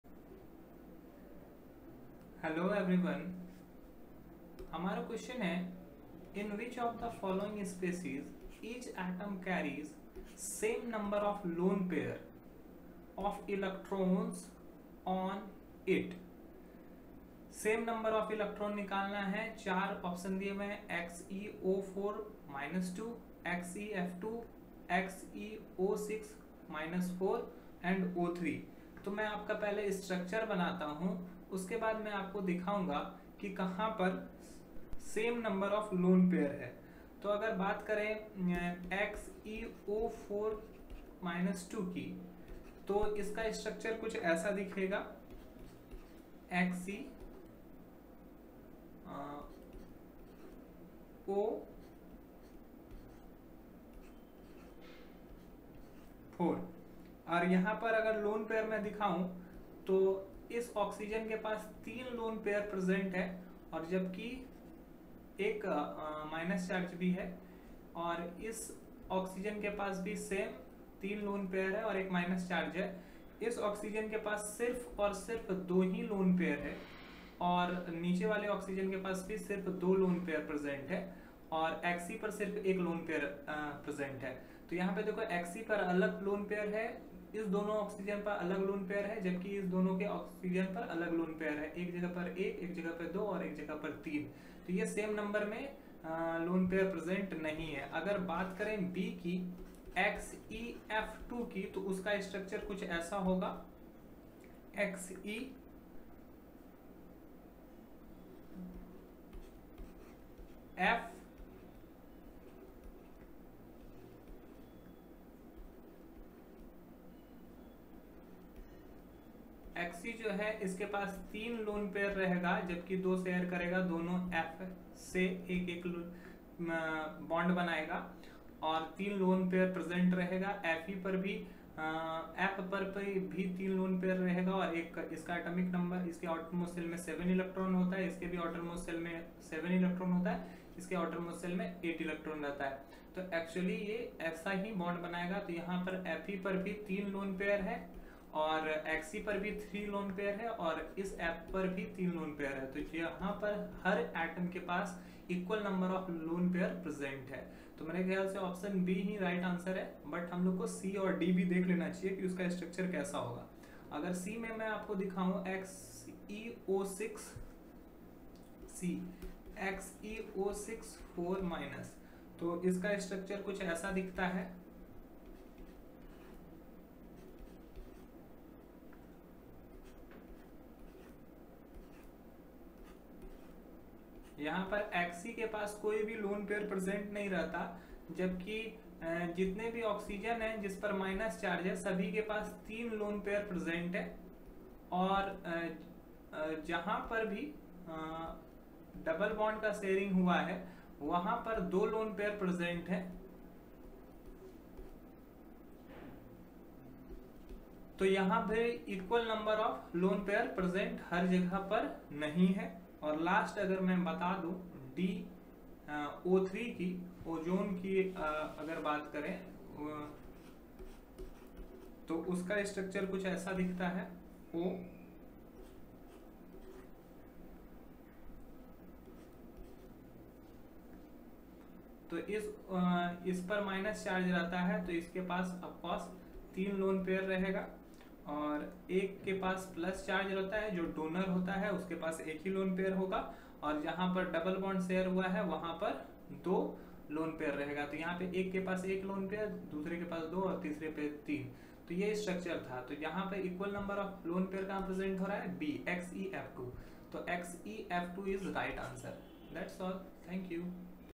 In which of the following species, each atom carries same number of loan pair of electron on it
00:00:00 In which of the following species, each atom carries same number of loan pair of electron on it